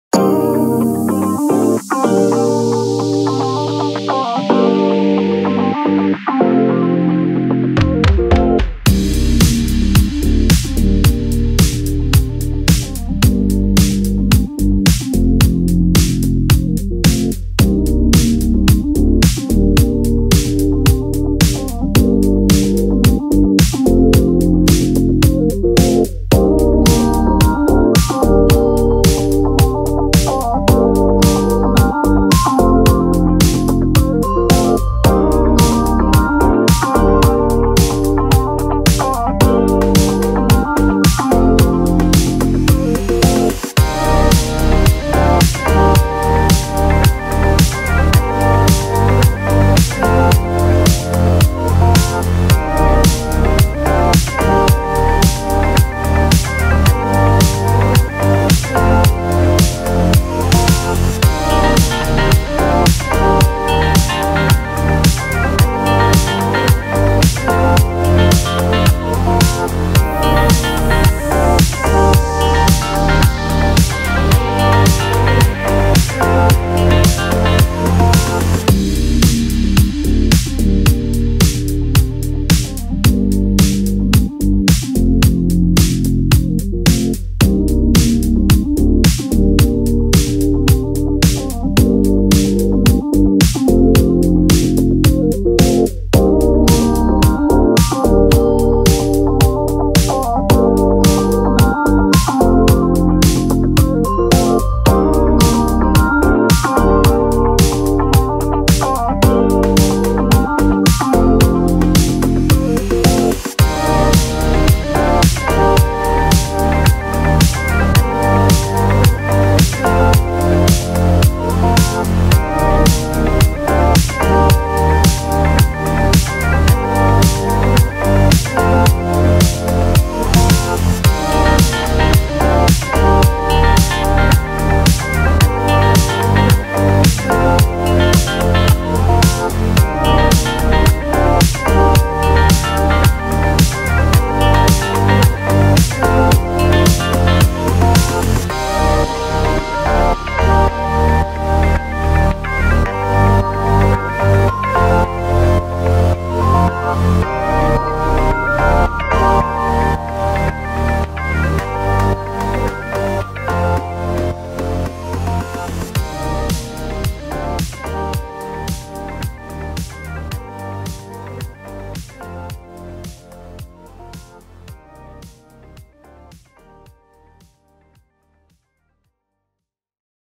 Casa de R$ 1.700.000,00 à venda - PORTEIRA FECHADA Residence Park - Granja Viana - Cotia/SP
Mais uma produção da Agência Social Film em parceria com a Portal Raposo Imóveis.